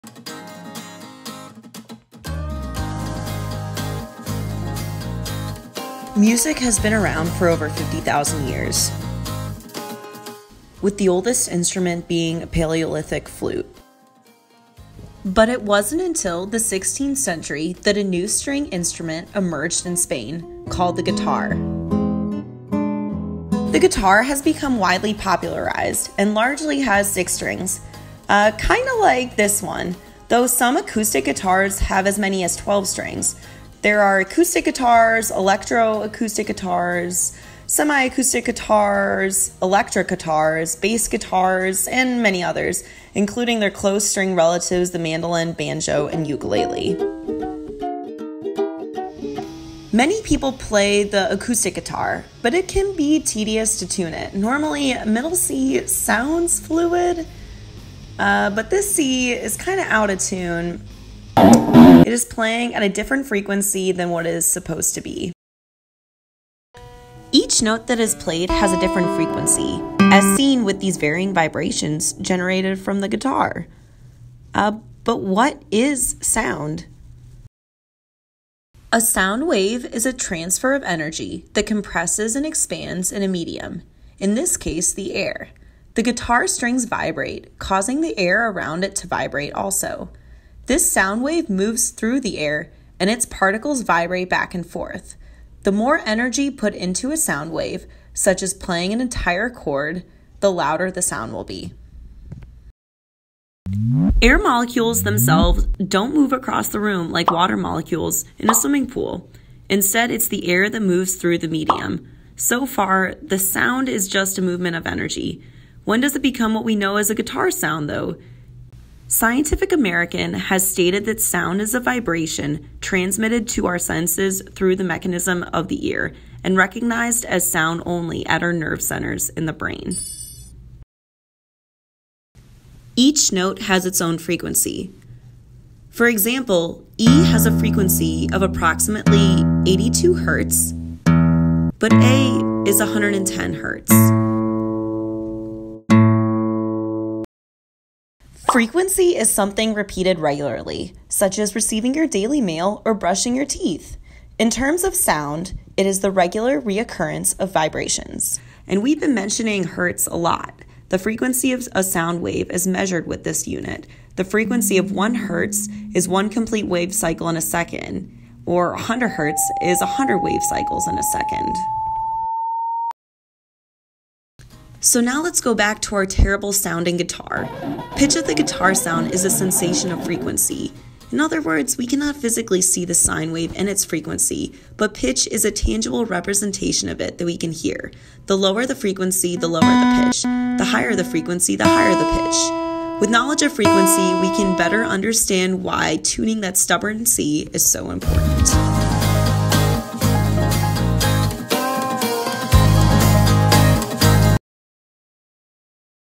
The Science of Frequency
We are launching our first science content video on our Youtube channel. Working in tune with the “Guitar” exhibit, which runs until April 14, the animated clip titled “The Science of Guitar Frequency” explains how frequency of guitar sounds are formed.